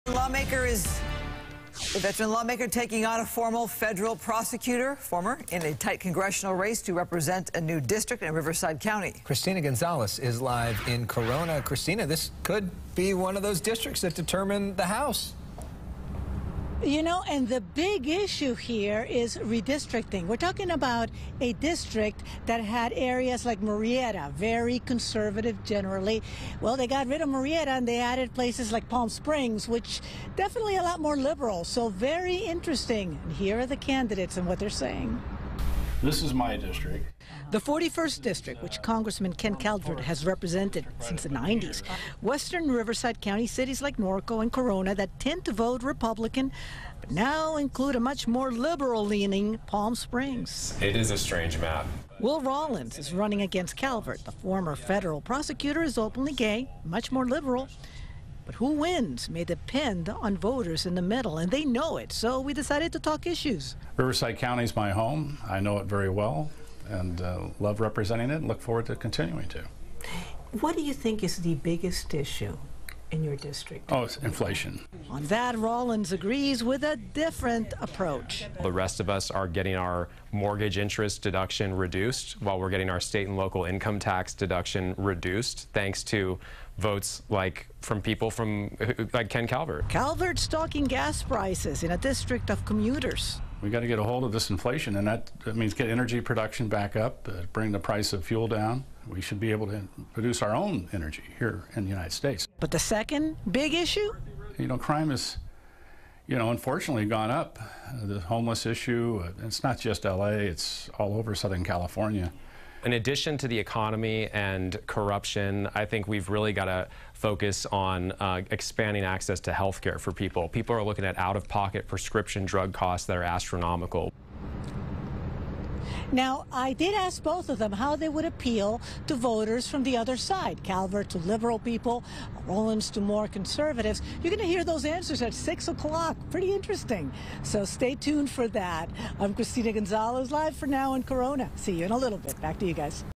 Race for District 41: Ken Calvert vs. Will Rollins
The campaign is in the home stretch for both Ken Calvert and Will Rollins, as they battle for votes in the seat for California's District 41.